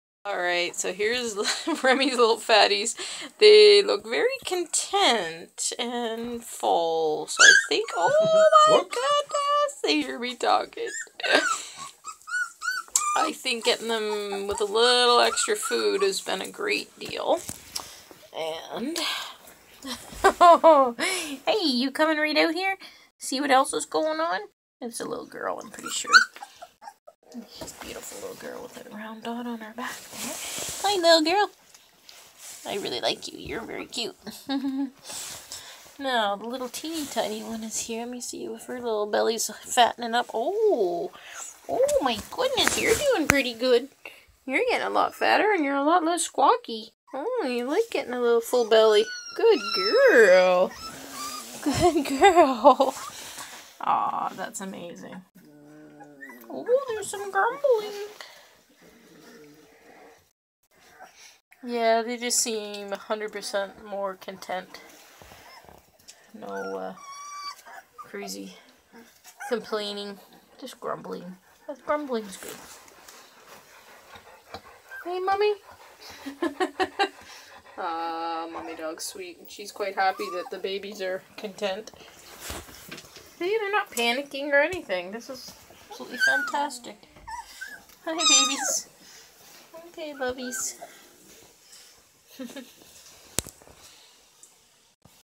Very content BLUETICK COONHOUND PUPPIES from Remington
These Bluetick puppies are eating some on their own plus drinking from mom.  They are very very content now!!

River Styx Scent Hounds in Ontario Canada.